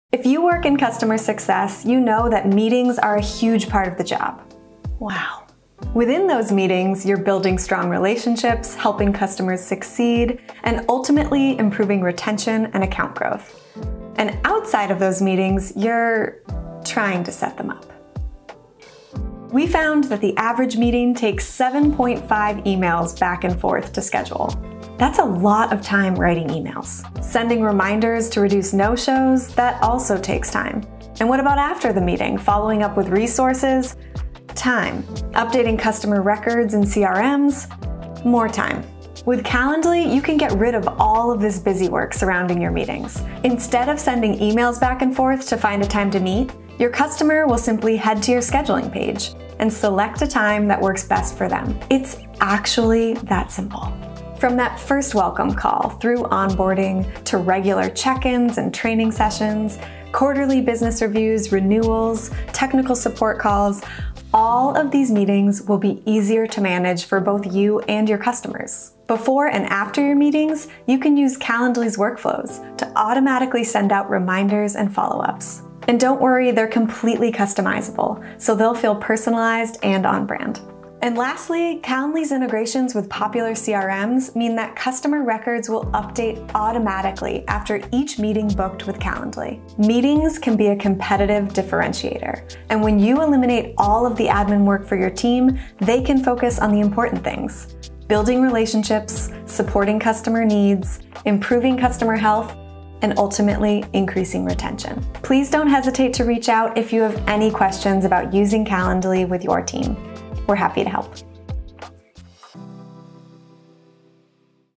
How to Use Calendly with Your Customer Success Team
Calendly is the easiest way to schedule, prepare, and follow-up on your meetings with customers. And we know you've got a LOT of meetings.

In this 2-minute video, we'll cover:

- Scheduling pages: Instead of sending multiple emails back and forth to find the best time to meet, share one link to a scheduling page (could be for an entire team or an individual), and your invitee will choose a time that’s convenient for them.
- Workflows: Automate all of the admin work around meetings (reminders, follow-ups, etc.)
- Integrations: Connect Calendly with tools you already use, like Salesforce, to automatically update lead records with meeting information.

Meetings can be a competitive differentiator, and when you eliminate all the admin work for your team, they can focus on what matters most: building relationships, supporting customer needs, improving customer health, and increasing retention.

To learn more about how Calendly could help your customer success team improve customer experience and satisfaction, head to this